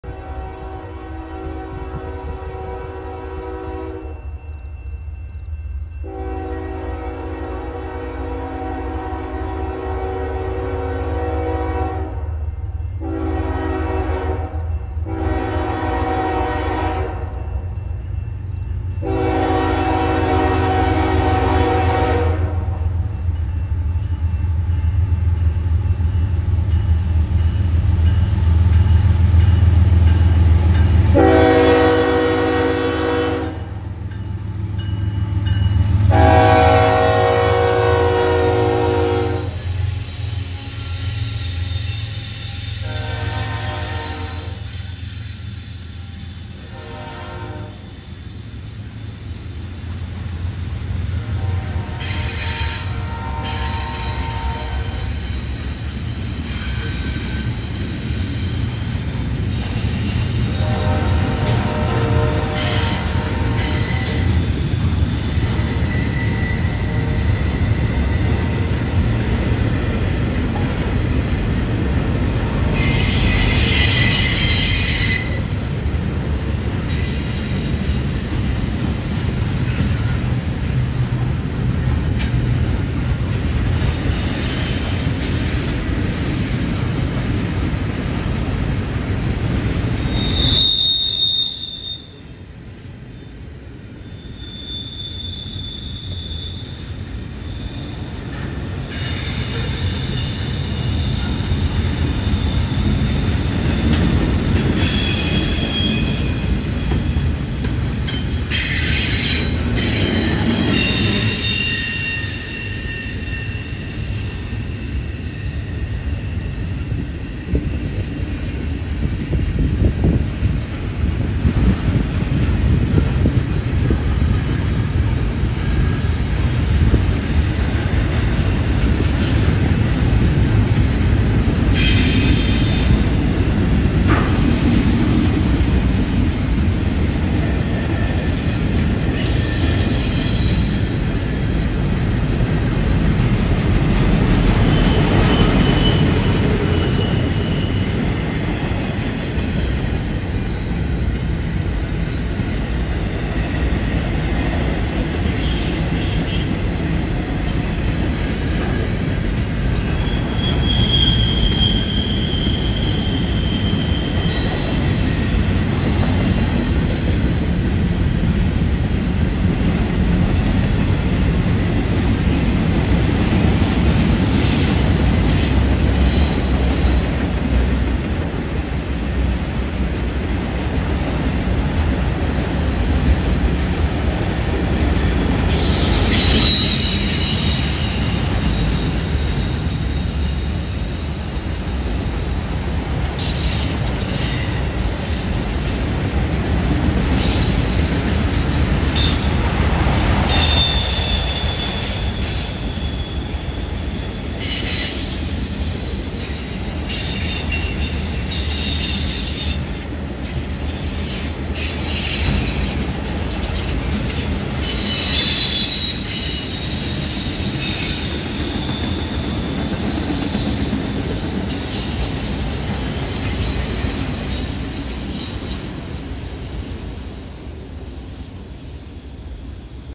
9/25/24 CN Train Z 149
9/25/24 05:21 63 degrees at CN MP 202 Charlotte as CN 3883 and CN 2953 power Train Z 149 over the curve. Full 3:50 video.